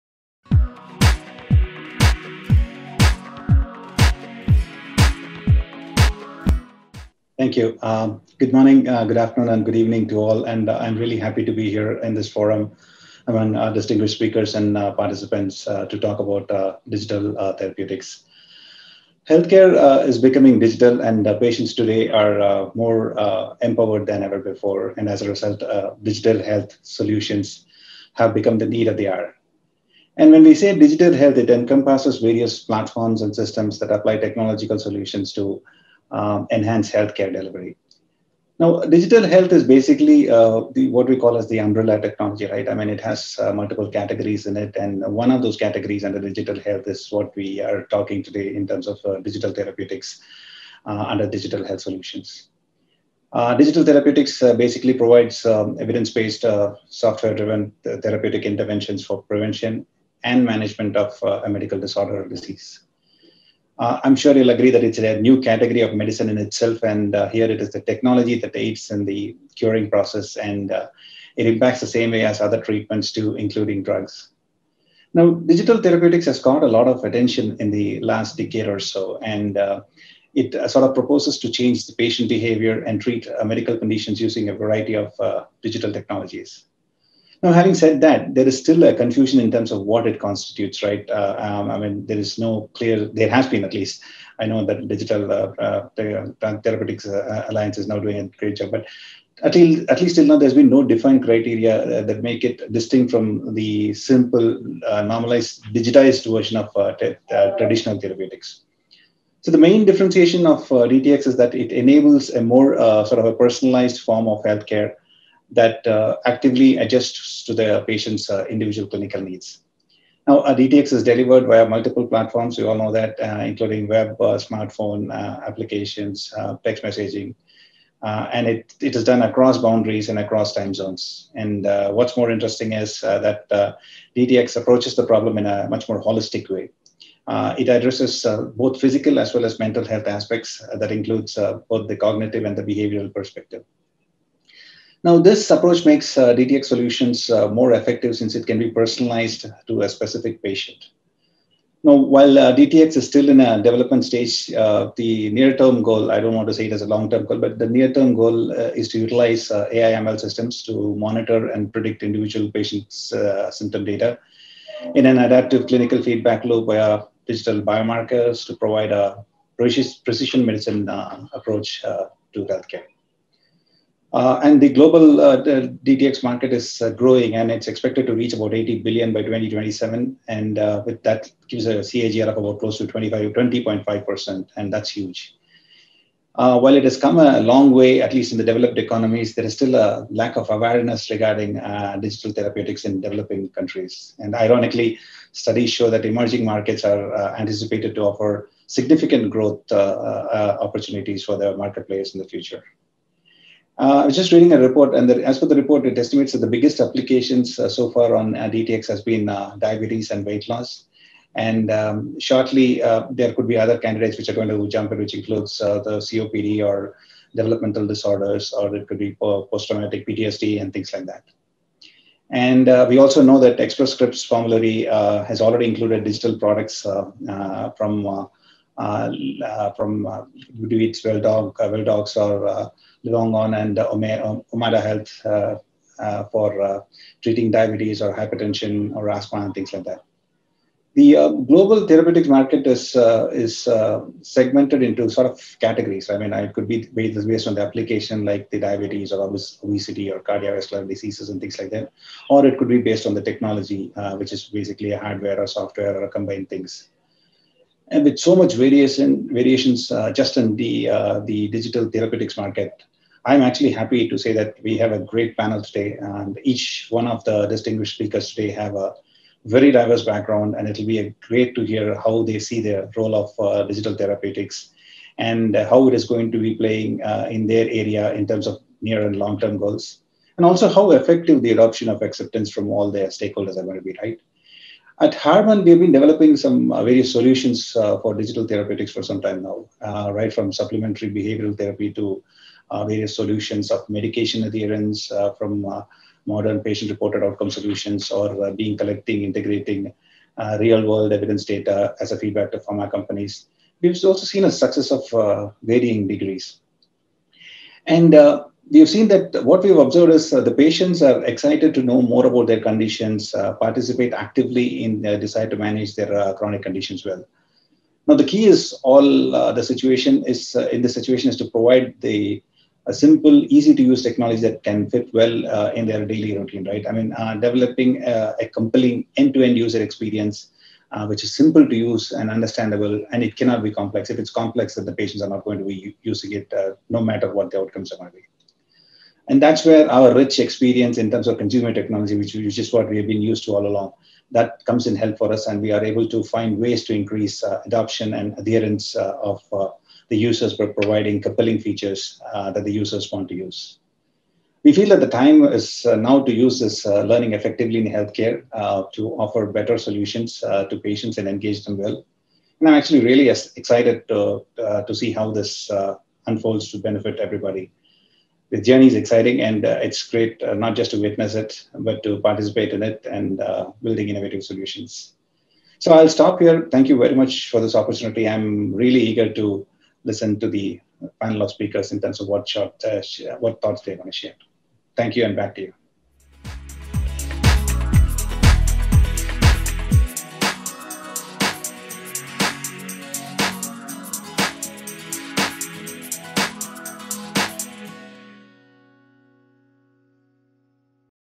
Digital Therapeutics: A New Category of Medicine | Keynote Talk
Healthcare stakeholders are increasingly deploying investments towards technologies such as AR/VR, AI/ML, sensors, platforms, etc., and are focusing on developing relevant digital capabilities to build DTx solutions. By providing access to real-world patient data for improved drug efficacy & individualized therapy, better patient engagement, and reduced cost of care, DTx is uniquely positioned to provide meaningful benefits across the spectrum of healthcare stakeholders.

In this keynote talk, Uma Shankar R, VP & GM, Digital Transformation Services, HARMAN talks about the digital therapeutics ecosystem, challenges and the benefits.